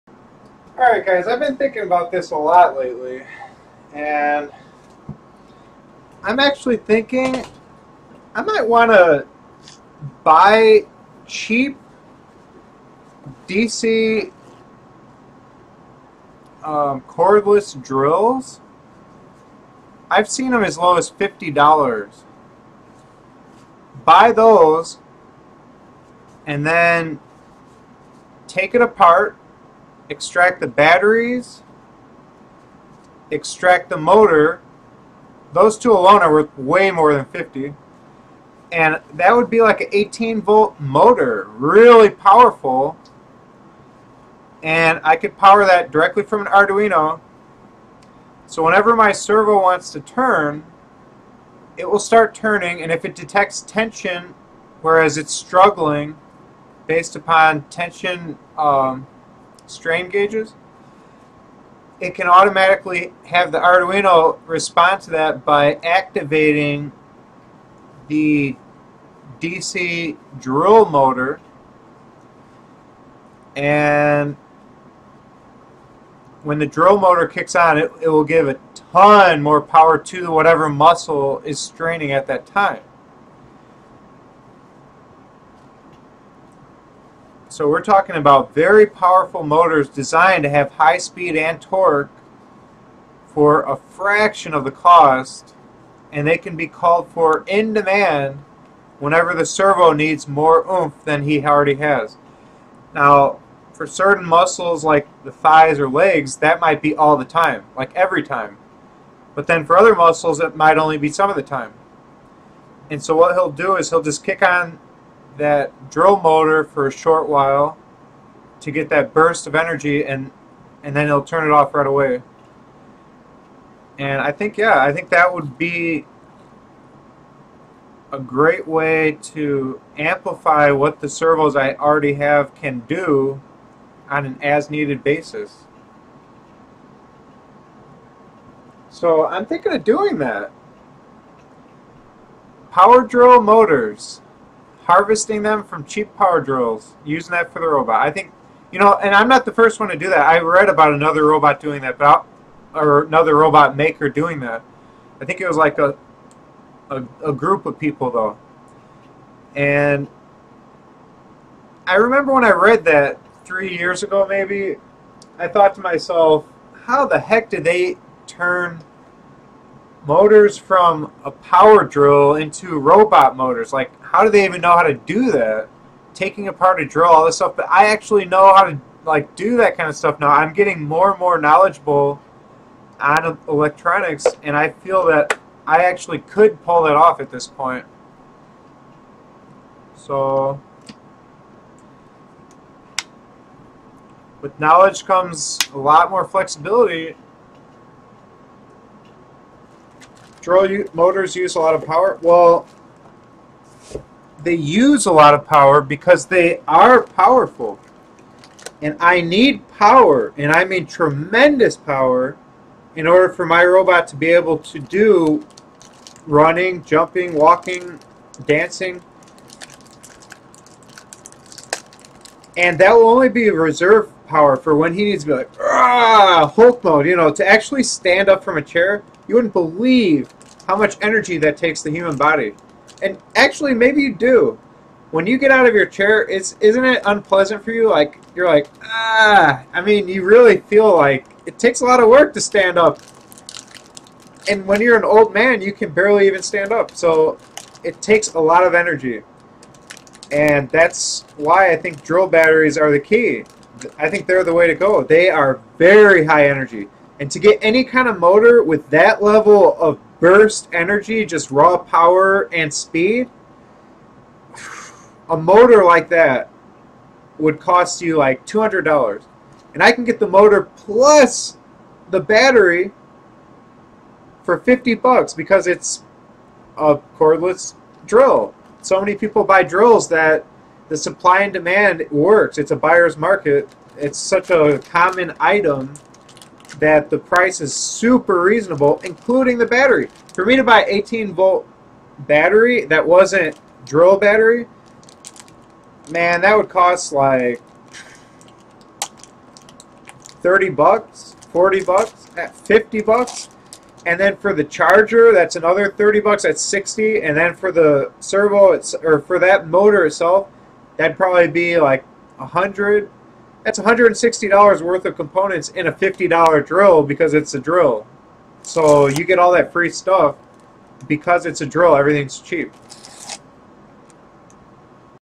Using Cordless Drill Motors for Largescale Robotics | Building Humanoid Robots #56
In this video, I discuss the strategy behind using cordless drill motors for my lifesized humanoid robot project.  They are cheap, they pack a powerful punch which I will need, they are both high in torque as well as fast, AND, they can be rigged up to be controlled by Arduino.  Plus, they can be used in demand to supplement standard hobby servos as needed.